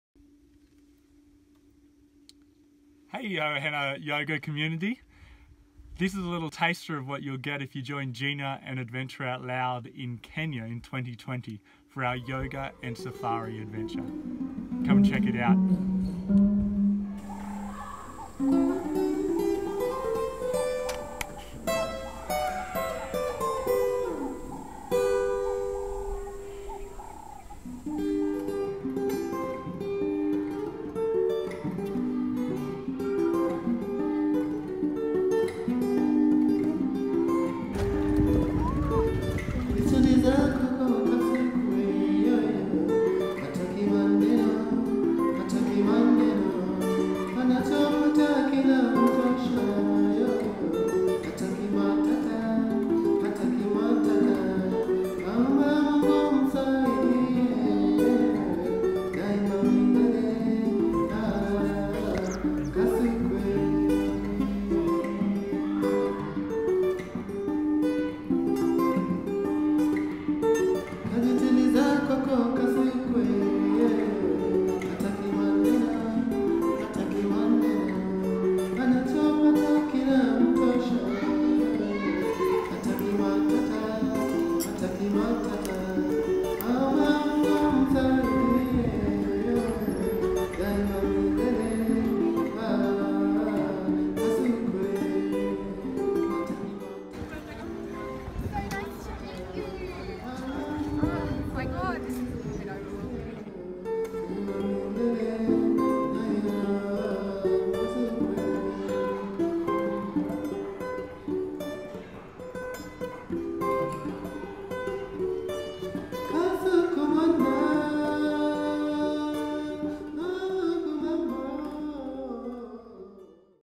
Gena Yoga Safari Adventure
If you want to get off the beaten track, immerse in local culture, make a difference in the world by sponsoring disadvantaged children through school and practice yoga as part of an epic African adventure... this yoga and safari adventure is for you! We're so excited to be teaming up with Gena Kenny from Ohana Yoga and we can't wait to see you in Kenya in 2020. More details